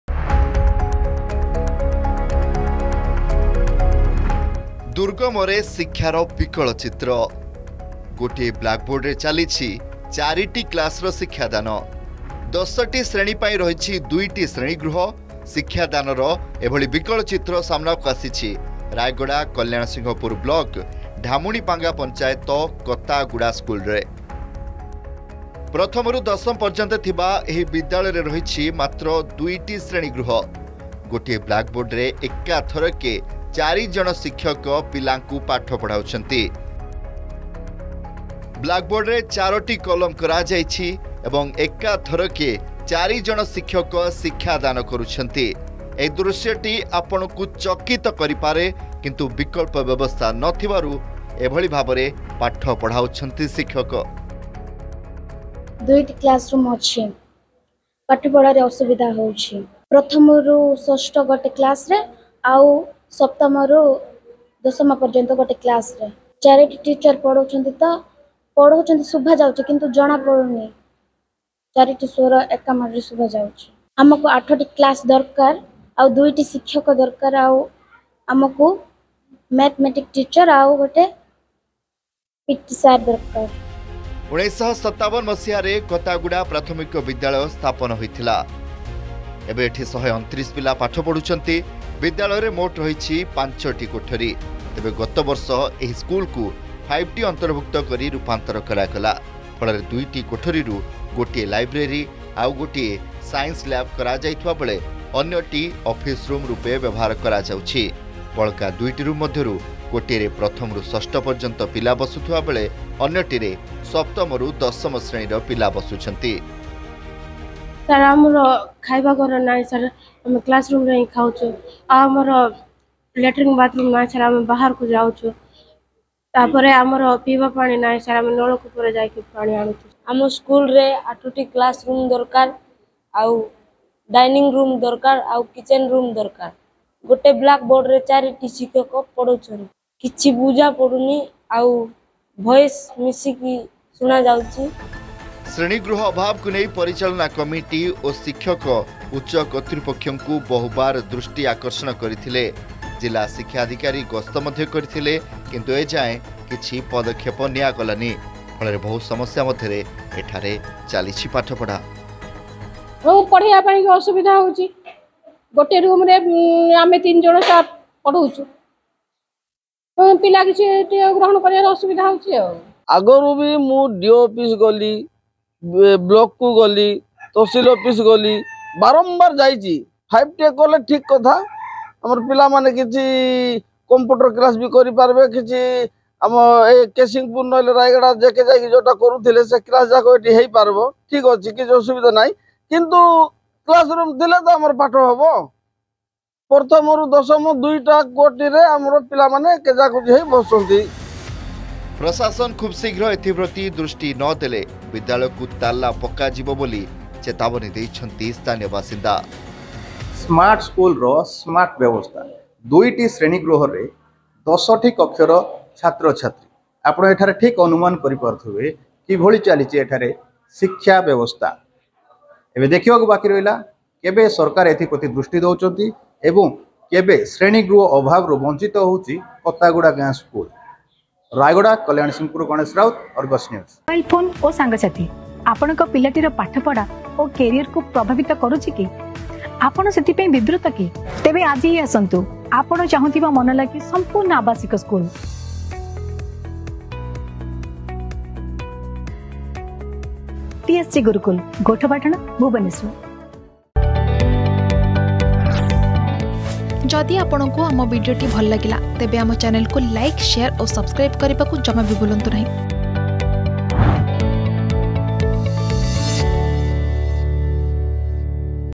ଦୁର୍ଗମରେ ଶିକ୍ଷାର ବିକଳ ଚିତ୍ର || School problem || Rayagada
ଭିତ୍ତିଭୂମି ବିହିନ ୫T ସ୍କୁଲ । ଦୁର୍ଗମରେ ଶିକ୍ଷାର ବିକଳ ଚିତ୍ର । ଗୋଟାଏ ବ୍ଳାକ୍ ବୋର୍ଡରେ ଚାଲିଛି ଚାରିଟି କ୍ଲାସ୍‌ର ଶିକ୍ଷାଦାନ । ୧୦ ଟି ଶ୍ରେଣୀ ପାଇଁ ରହିଛି ଦୁଇଟି ଶ୍ରେଣୀଗୃହ ।